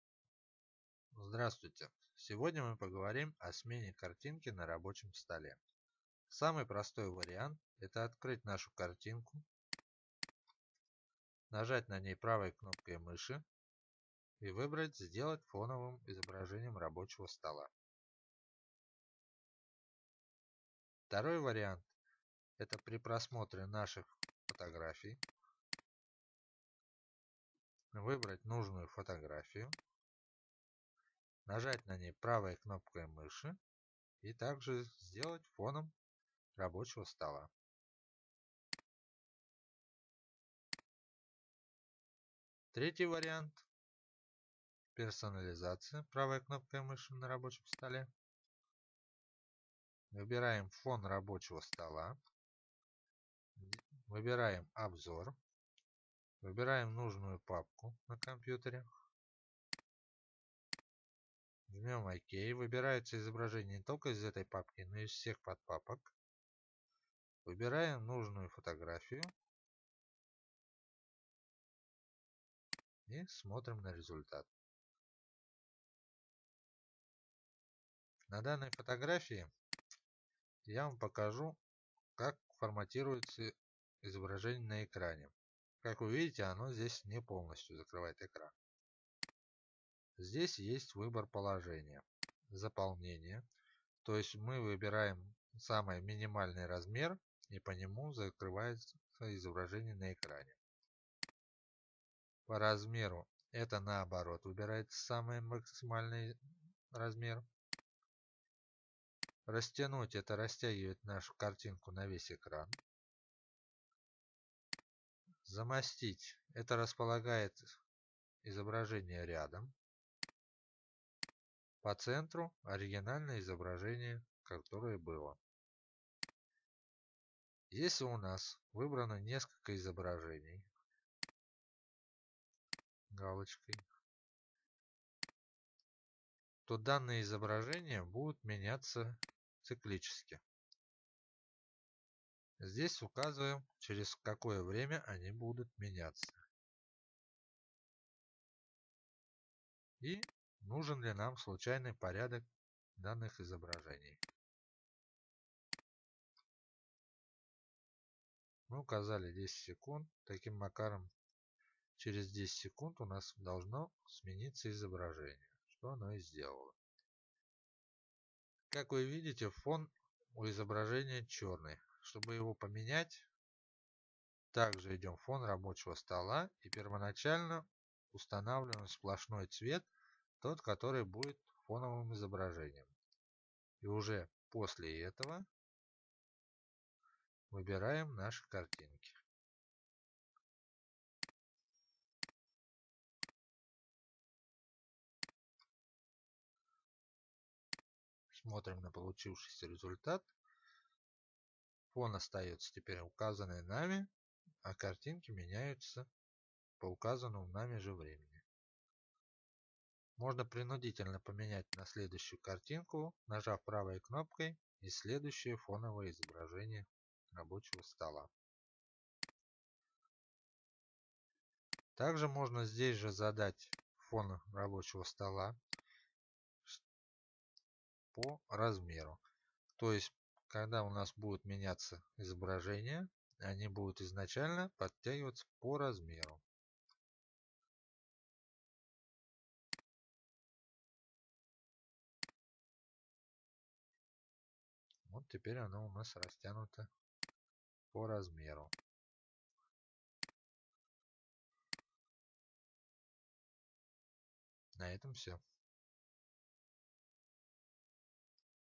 Замена фоновой картинки на рабочем столе
Замена фоновой картинки на рабочем столе в Windows 7